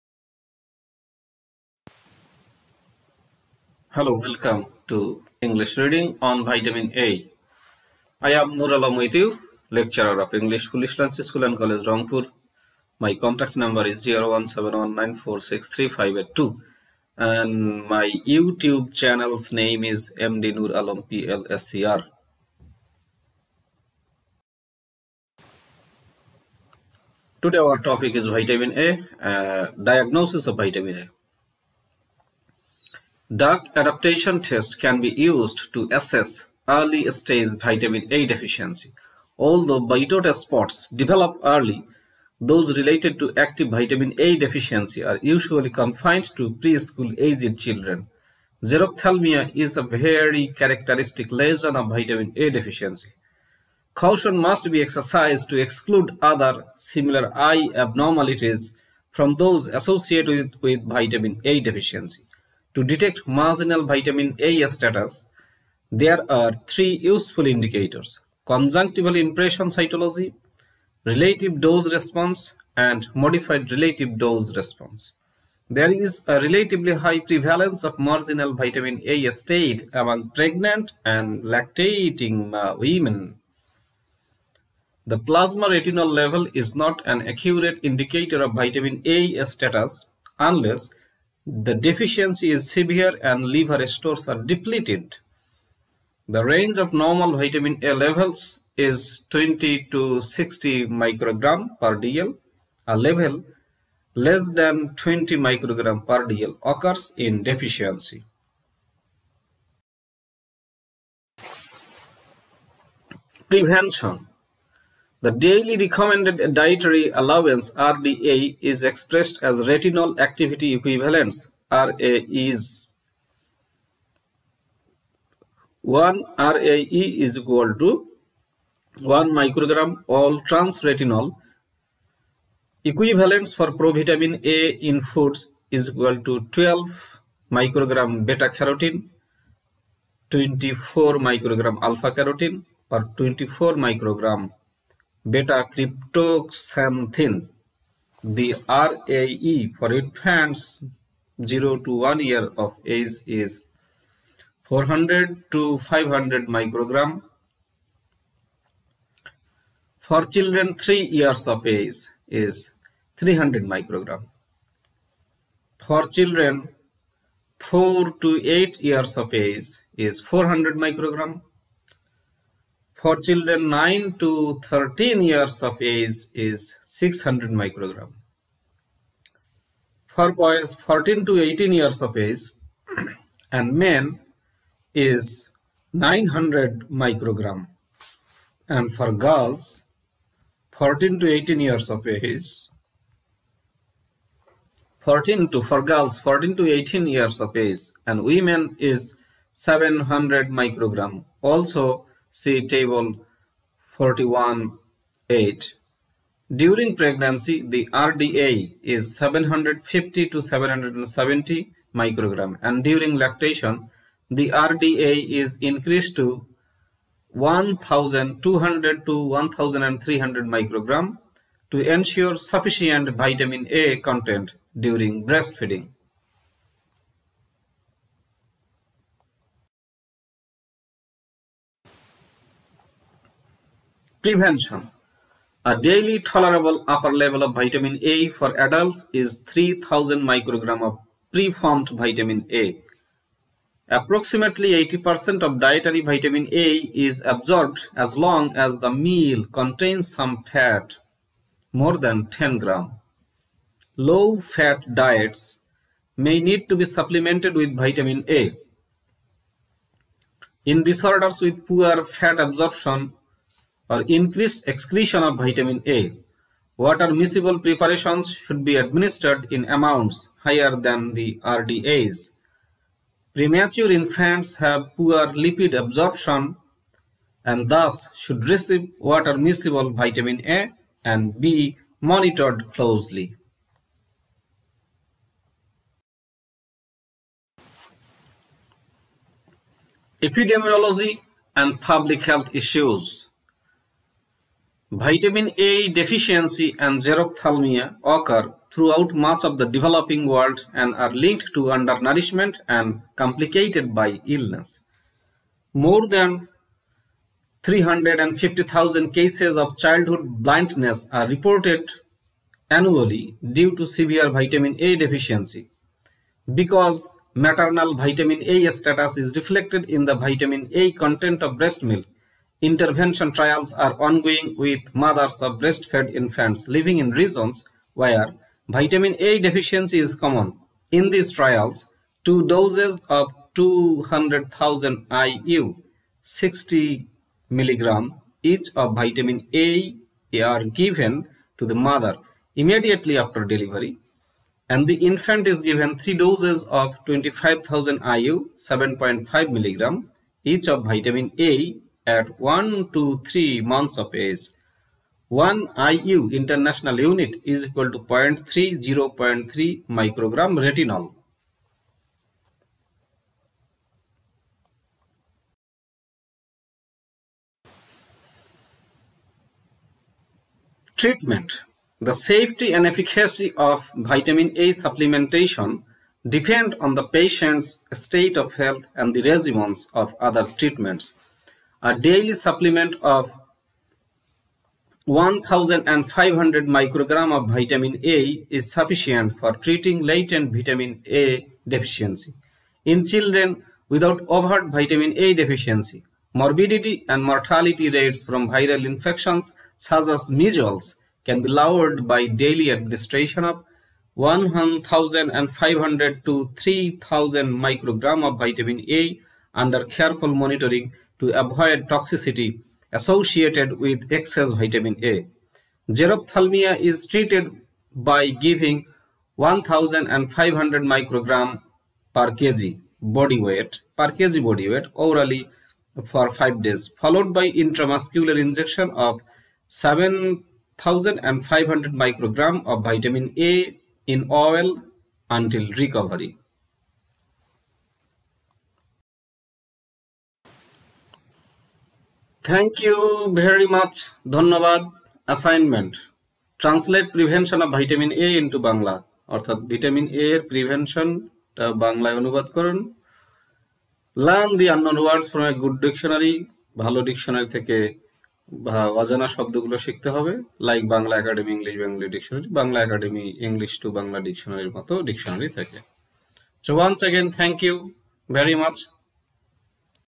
VITAMIN-A -DIAGNOSIS, PREVENTION AND TREATMENT
Students will be able to know about more up to date features of Vitamin A like

Vitamins being very requiring in human body should be extensively perused by students so that knowledge on VITAMIN becomes a part of our culture and so that no one in Bangladesh suffers from the paucity of vitamin.
After reading the text, students can know about Vitamin A and can hone their English reading skill . Thus, they can diversify their knowledge in English.